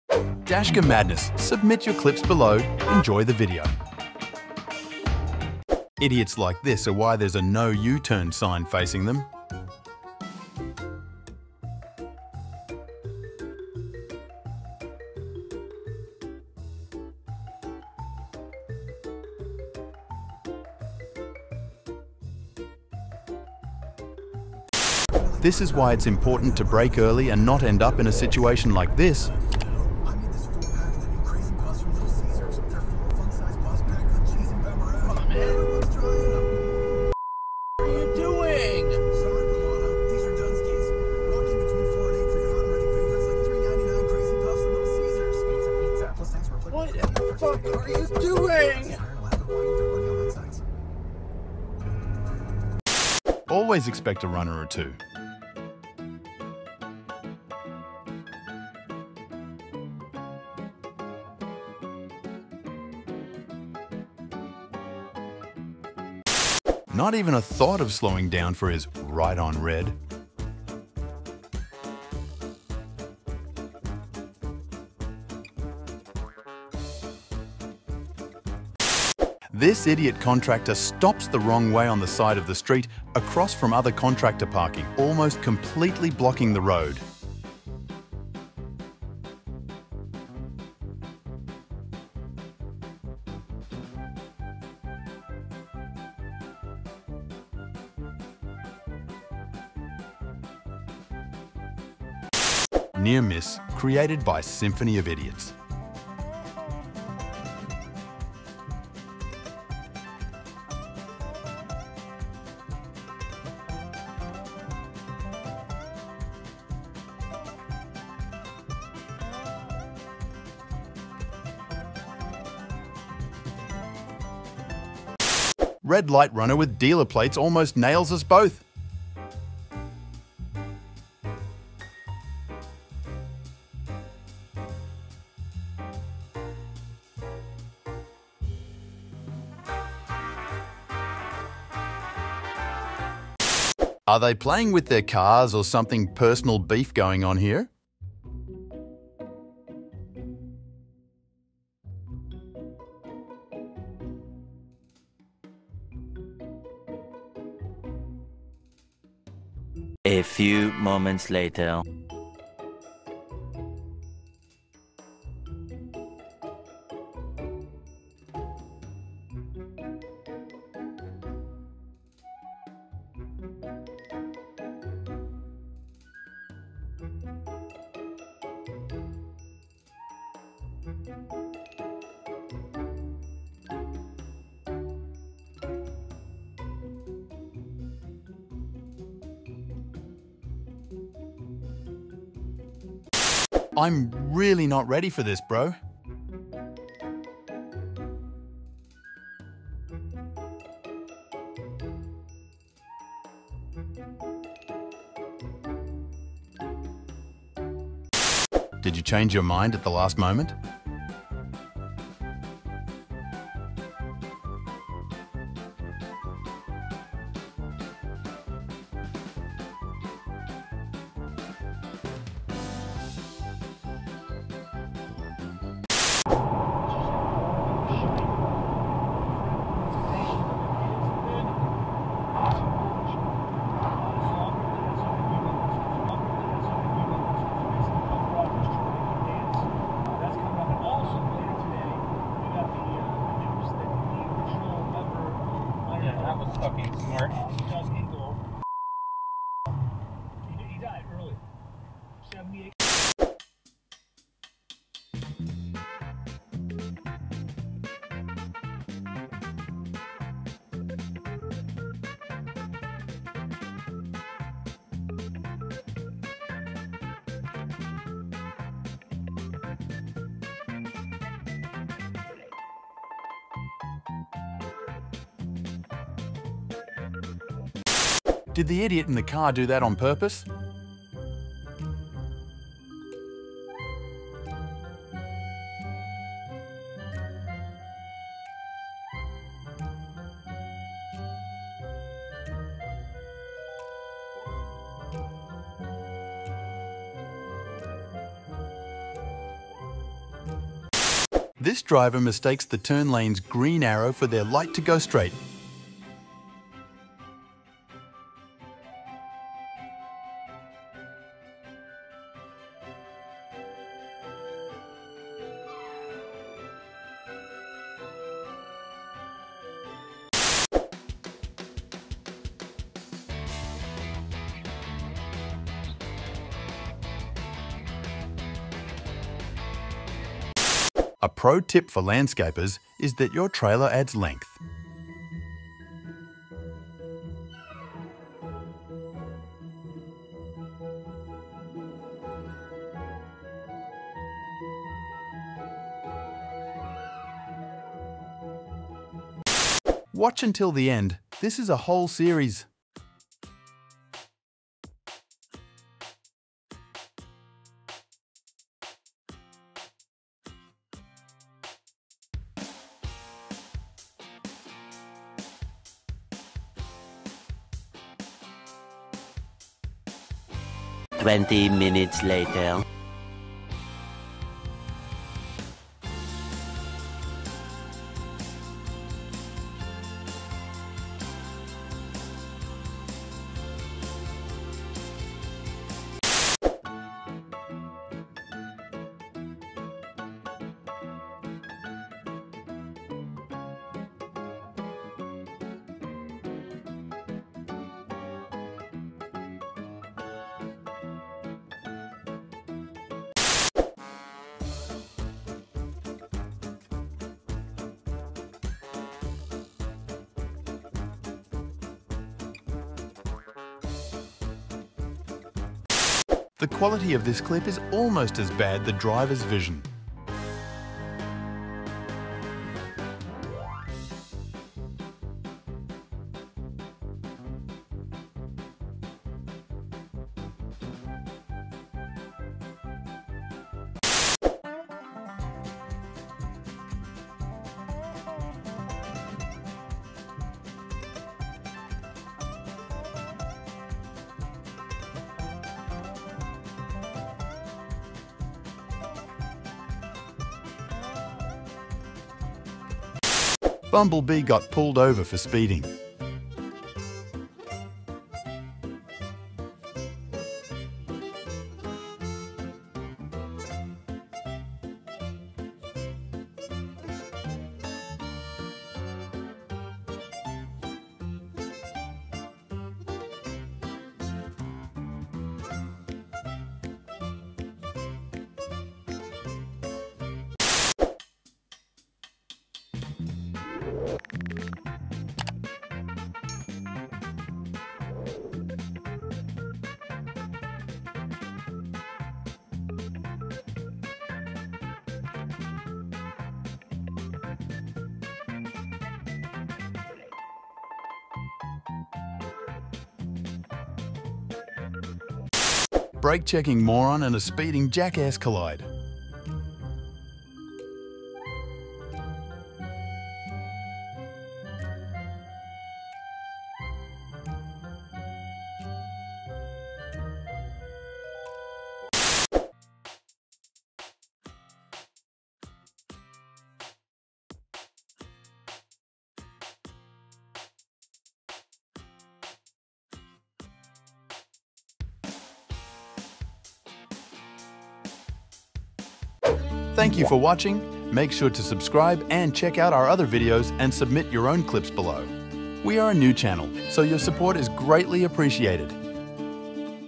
IDIOT DRIVER Runs From Cops & NEARLY CRASHES
Crazy Idiot Drivers Caught on Dash Cam

In this video, you will be watching a Dashcam Compilation of Funny Moments, Car crashes, Driving Fails, Near Misses, and Karma!

This footage can be used as an education guide on how to prevent car accidents and near crashes!

Have you ever been in a Near Head on Collision, Hit & Run or Emergency Situation?